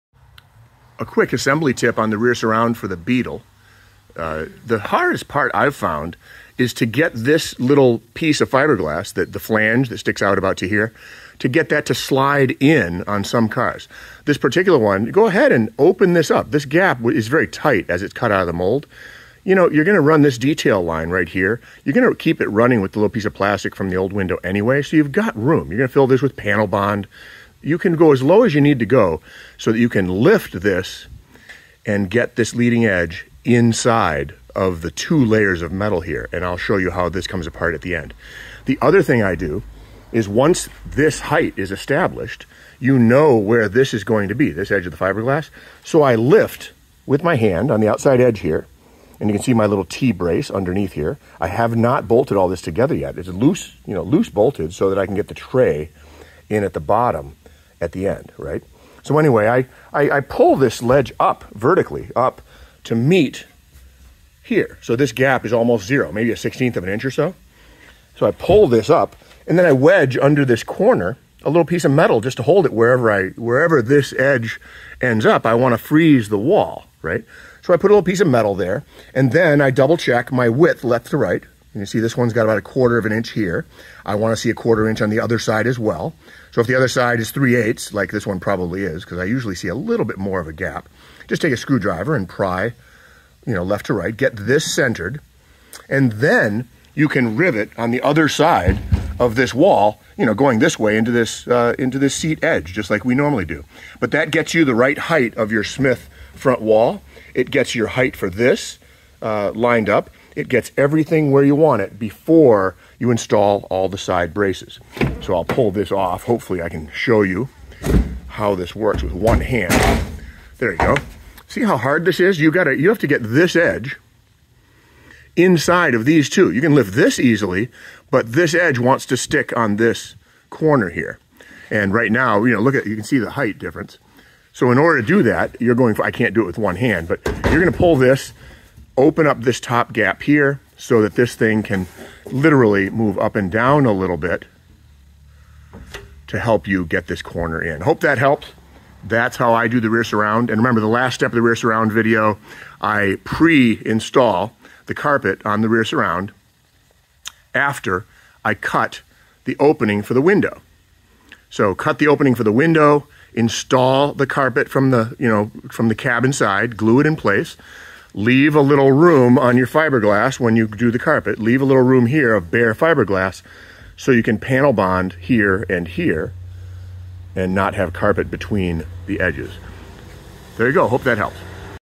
Beetle ute rear window surround install tips.
Install the rear surround. Lift the front aluminum wall to meet it, and then finalize all of your mounting spots. A quick video reviewing the method I use.